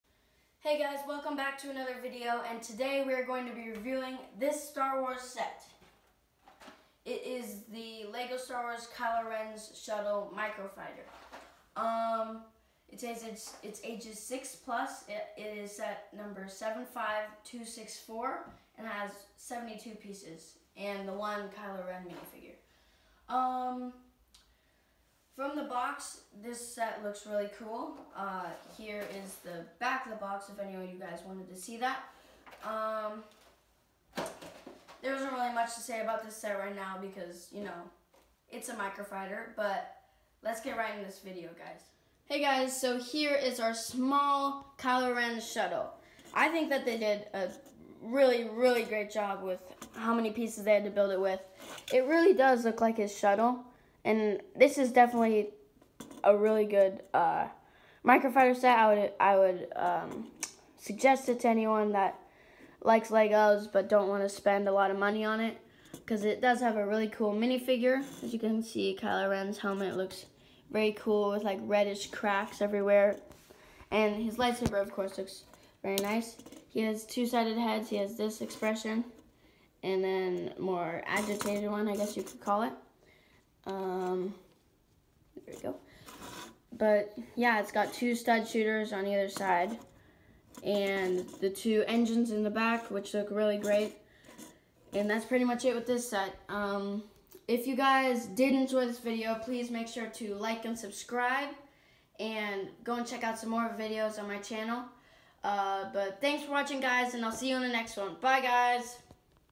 Lego Star Wars Kylo Ren's Micro fighter
This video is a review on the series 7 Kylo ren's Micro fighter